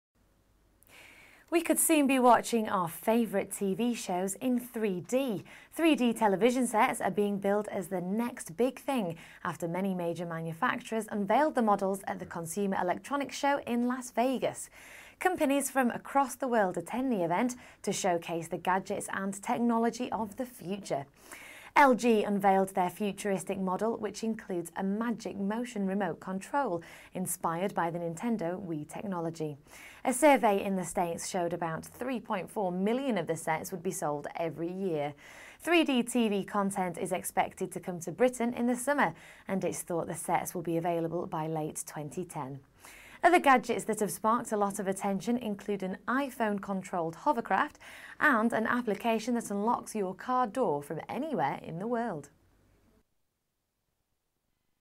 3D TV could come to Britain by the end of next year
We could soon be watching our favourite TV shows in 3D.

3D television sets are being billed as the next big thing after many major manufacturers unveiled the models at the Consumer Electronics Show in Las Vegas.

Companies from across the world attend the event to showcase the gadgets and technology of the future.

LG unveiled their futuristic model which includes a Magic Motion remote control, inspired by the Nintendo Wii technology.

A survey in the States showed about 3.4million of the sets would be sold every year.

3D TV content is expected to come to Britain in the summer and its thought the sets will be available by late 2010.

Other gadgets that have sparked a lot of attention include an iPhone controlled hovercraft and an application that unlocks your car door from anywhere in the world.